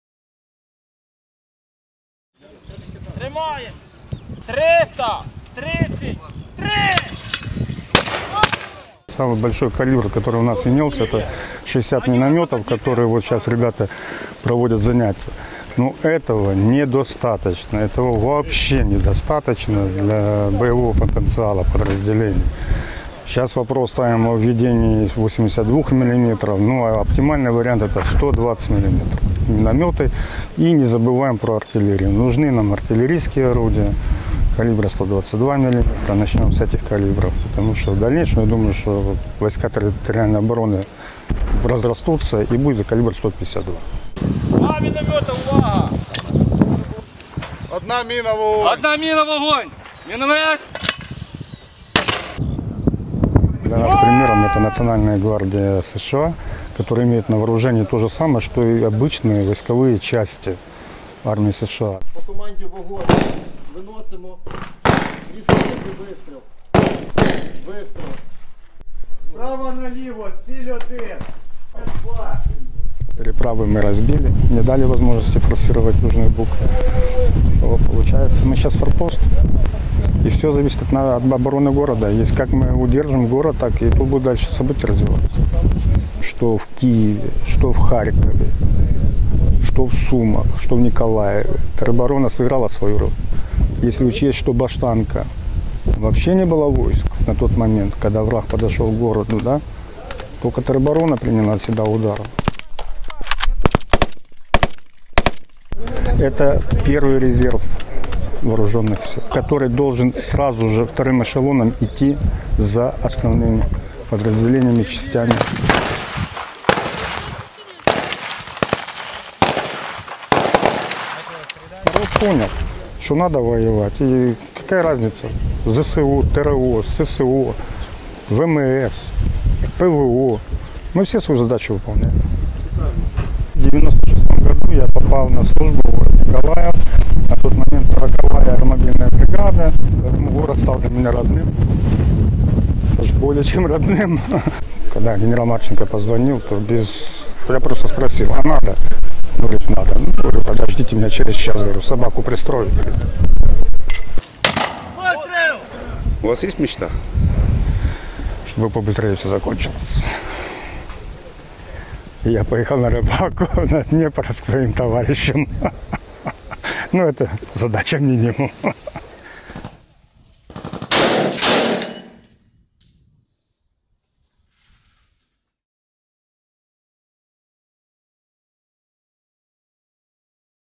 ТРО - не пістолетні війська
Що спільного має бути у ТРО і Нацгвардії США?

Джерело: Сили територіальної оборони ЗСУ